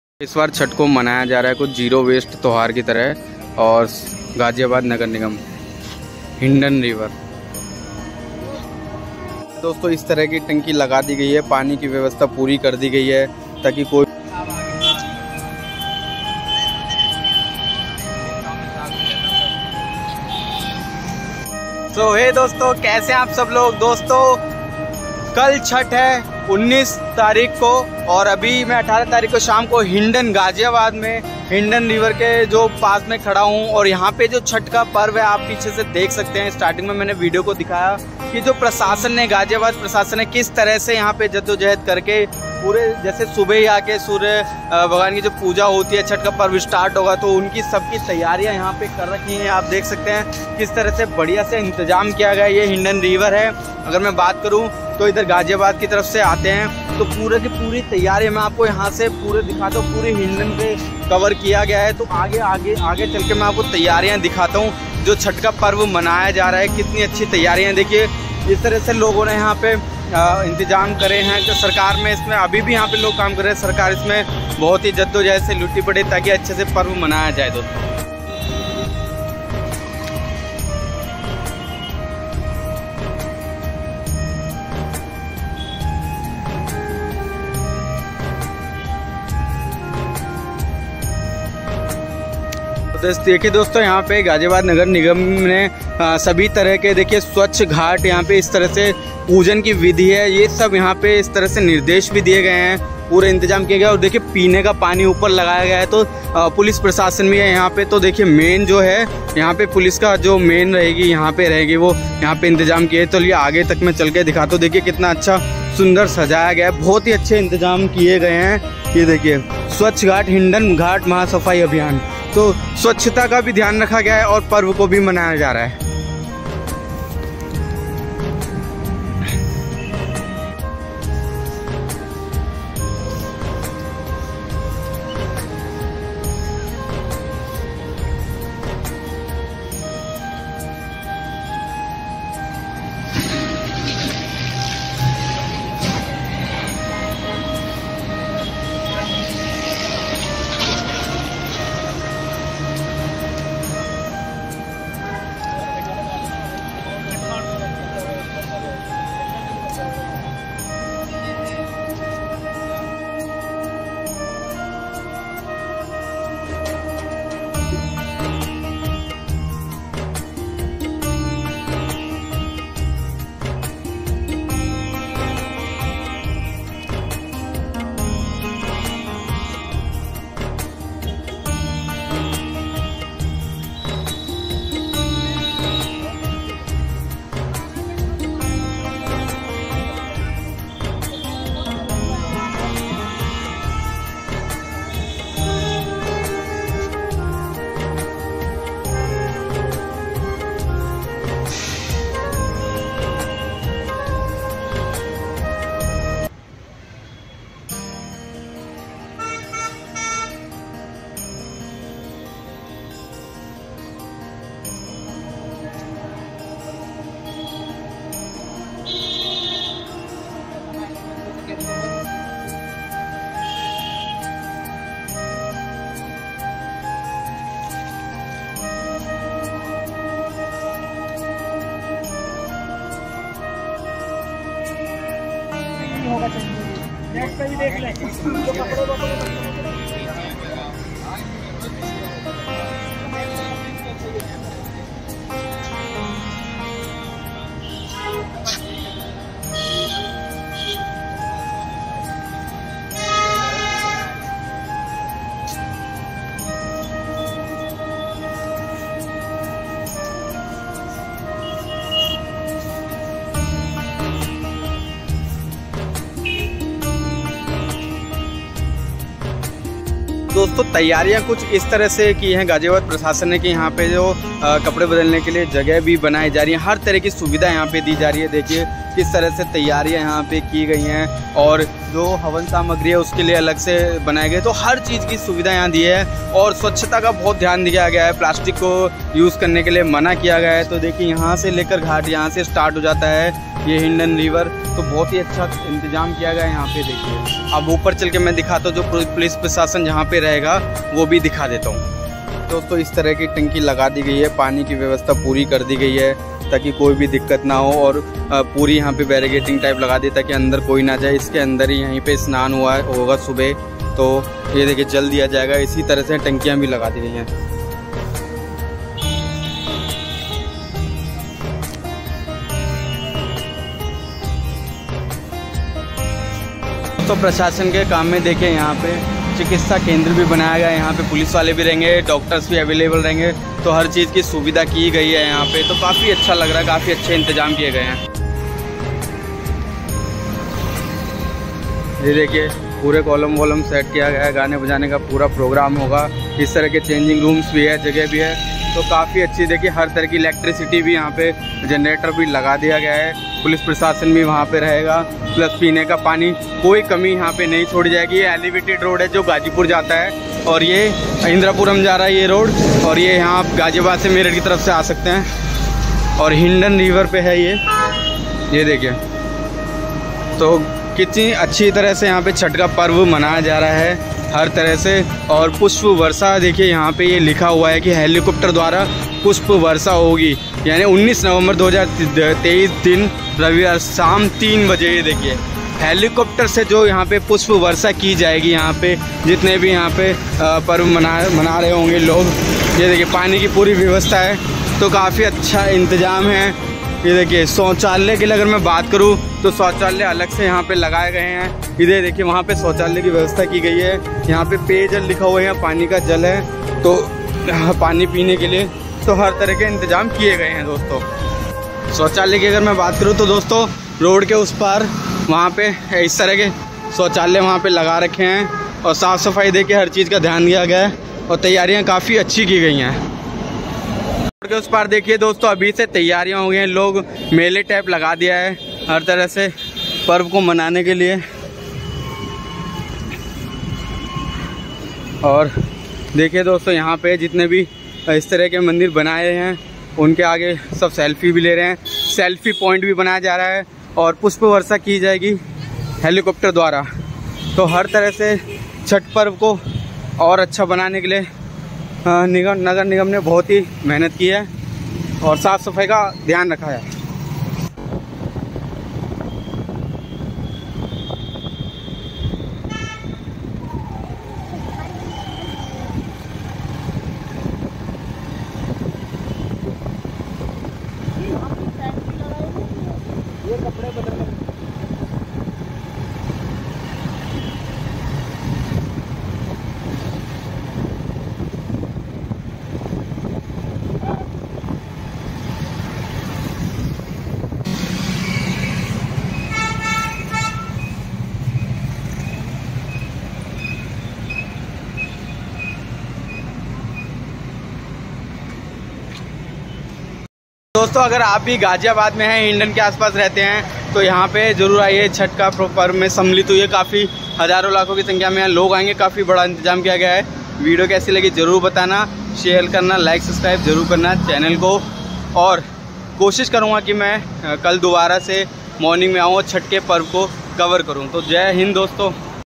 छठ पूजा का सबसे बड़ा इंतजाम हिंडन नदी ग़ाज़ियाबाद, हेलीकाप्टर से होगी पुष्प वर्षा
Chhath Puja is a Hindu festival dedicated to the worship of the Sun God, Surya, and Chhathi Maiya, the Goddess of Chhath. Celebrated primarily in the Indian states of Bihar, Jharkhand, Uttar Pradesh, and in some parts of Nepal, it usually takes place six days after Diwali. Devotees observe a rigorous four-day ritual that involves fasting, holy bathing, and offering prayers to the rising and setting sun. The festival is marked by rituals performed at ghats (riverbanks) and involves family and community participation. It is a significant cultural event that showcases reverence for nature and the Sun as a life-giver.